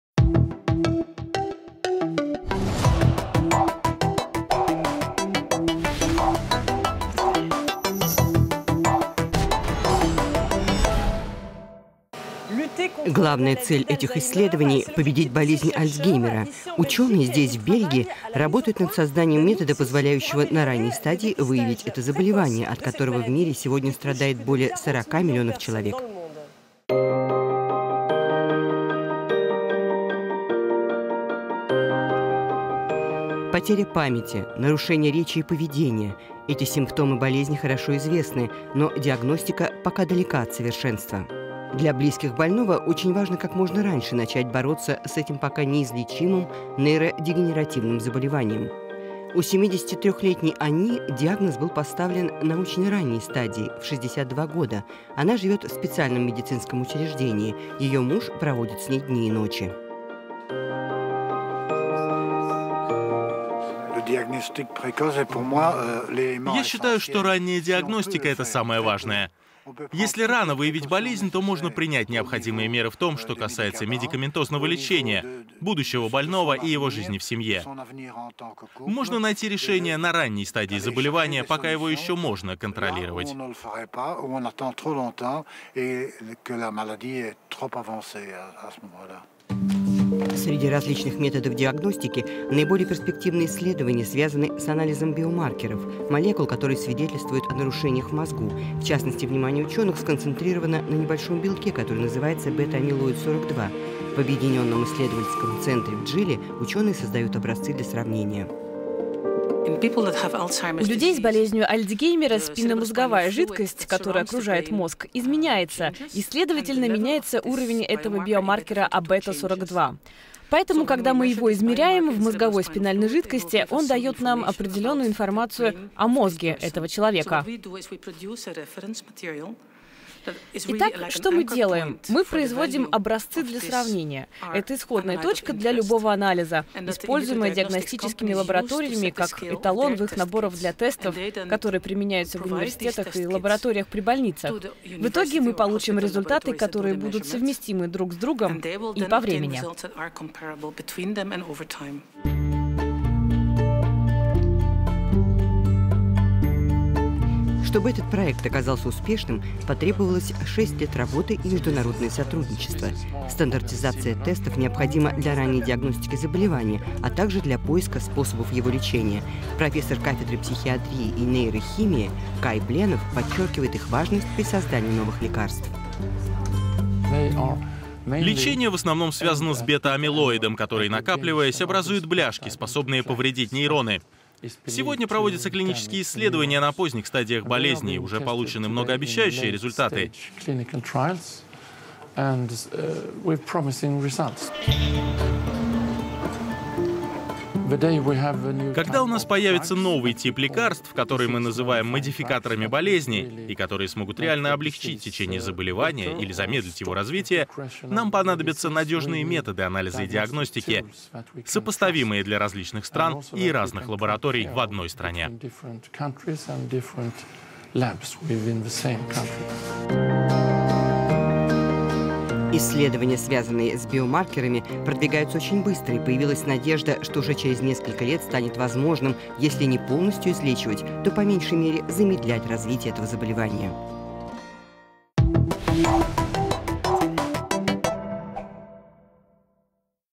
Альцгеймер - не приговор? Ученые на пороге создания лекарства - futuris
Как диагностировать болезнь Альцгеймера на раннем этапе, чтобы замедлить ее развитие и облегчить жизнь пациентов? Наконец, в исследованиях наметился прогресс.

Анн Девино, евроньюс: "Главная цель этих исследований – победить болезнь "Альцгеймера" Ученые, здесь, в Бельгии, работают над созданием метода, позволяющего вовремя выявить это заболевание, от которого в мире страдают более 40 миллионов человек".